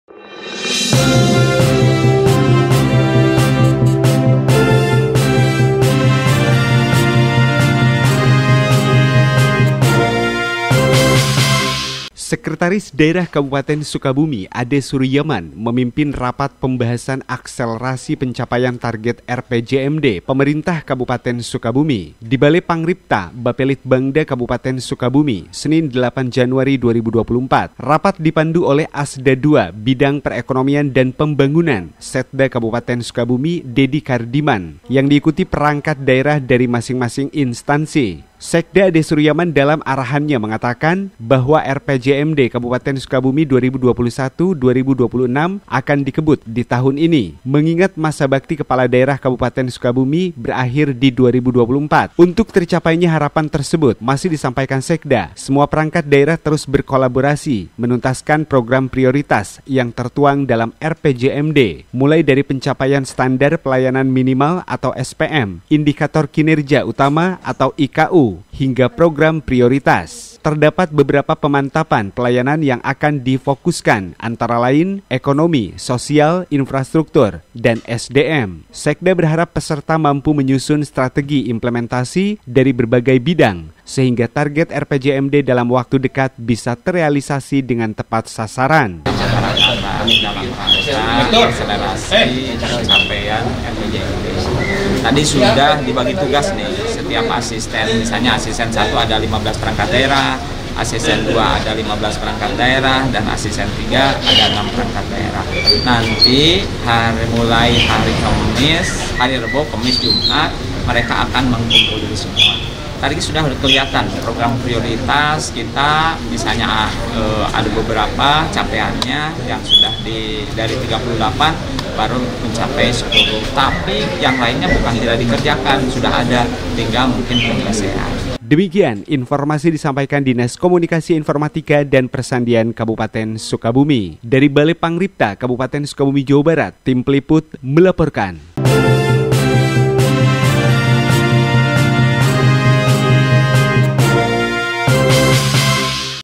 RAPAT PEMBAHASAN AKSELERASI PENCAPAIAN TARGET RPJMD
Sekretaris Daerah Kabupaten Sukabumi Ade Suryaman memimpin Rapat Pembahasan Akselerasi Pencapaian Target RPJMD Pemerintah Kabupaten Sukabumi, di Bale Pangripta, Bappelitbangda Kabupaten Sukabumi, Senin, 8 Januari 2024. Rapat dipandu oleh Asda II Bidang Perekonomian dan Pembangunan, Setda Kab. Sukabumi, Dedi Chardiman, yang diikuti perangkat daerah dari masing-masing instansi.

Sekda Ade Suryaman dalam arahannya mengatakan, bahwa RPJMD Kabupaten Sukabumi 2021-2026 akan dikebut ditahun ini. Mengingat masa bakti kepala daerah Kabupaten Sukabumi berakhir di 2024.

"Berdasarkan hasil keputusan, bahwa Pak Bupati akan berakhir pada tanggal 31 Desember 2024. Jadi capaian RPJMD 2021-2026 mesti rampung ditahun ini," tegasnya.

Untuk tercapainya harapan tersebut sambung Sekda, semua perangkat daerah terus berkolaborasi menuntaskan program prioritas yang tertuang dalam RPJMD, mulai dari pencapaian Standar Pelayanan Minimal (SPM), Indikator Kinerja Utama (IKU), hingga program prioritas.

Terdapat beberapa pemantapan pelayanan yang akan difokuskan antara lain, ekonomi, sosial, infrastruktur dan SDM.

Sekda berharap, peserta  mampu menyusun strategi implementasi dari berbagai bidang,  sehingga target RPJMD dalam waktu dekat bisa terealisasi dengan tepat sasaran.

"Dengan rapat ini diharapkan dapat menyelaraskan persepsi rencana pencapaian target program prioritas di 2024," singkatnya.